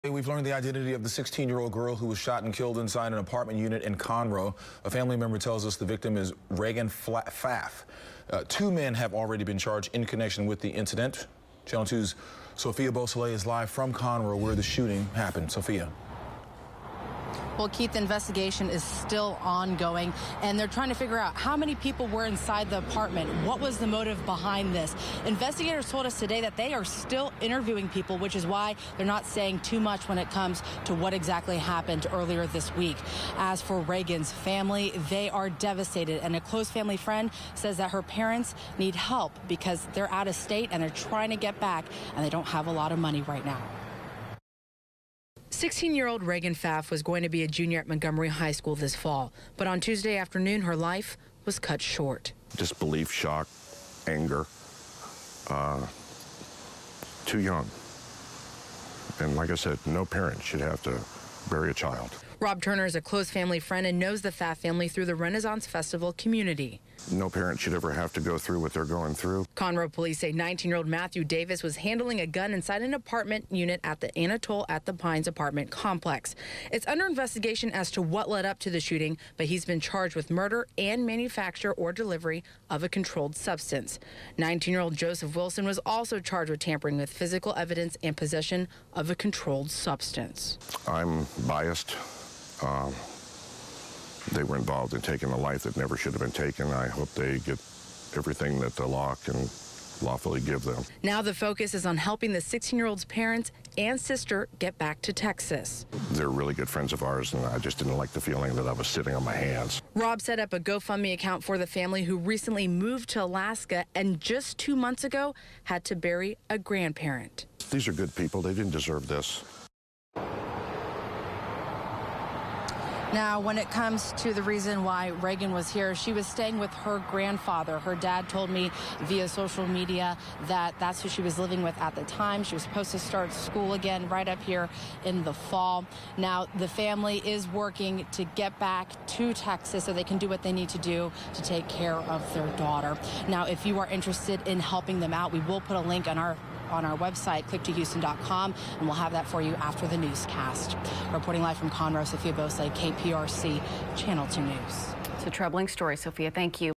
Family friend remembers teen killed
A 16-year-old girl who was shot and killed inside an apartment unit in Conroe has been identified.